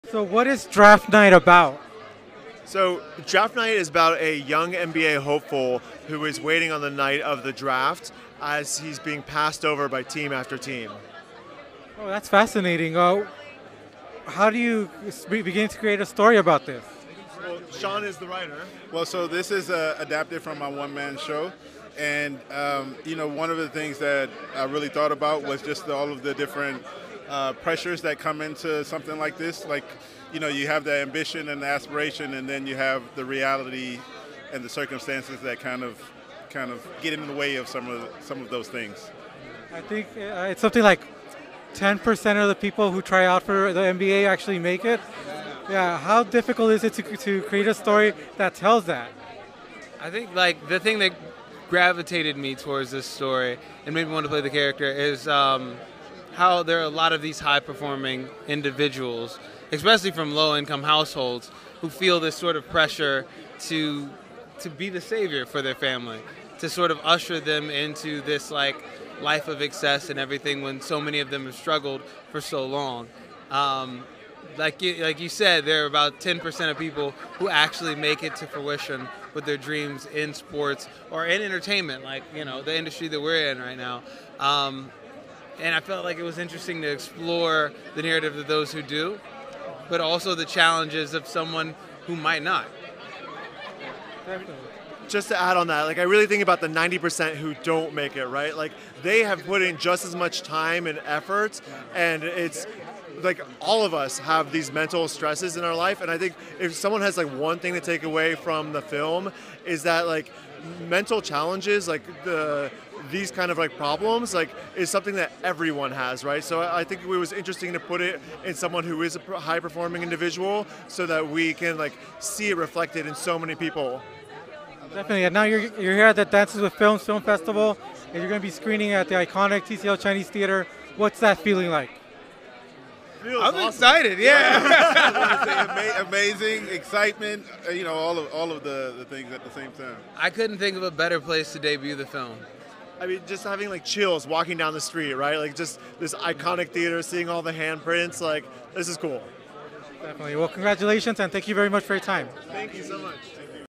DRAFT NIGHT’s Cast, Filmmakers at the red carpet for DWF 2024
Jesus Figueroa, ​⁠​⁠​⁠​⁠ of ThisFunktional.com, talks with Writer/Actor Shawn Williams, Actor Joseph David-Jones, Director Thomas Glinkowski about DRAFT NIGHT at the red carpet for DWF 2024.

A young Black basketball prodigy struggles with expectations, pressure, and the weight of his own ambition as he waits to hear his name called on the night of the NBA draft.
Anchor podcasts:
anchor.fm/ThisFunktional
anchor.fm/while-we-were-gaming
anchor.fm/LatinX-Talks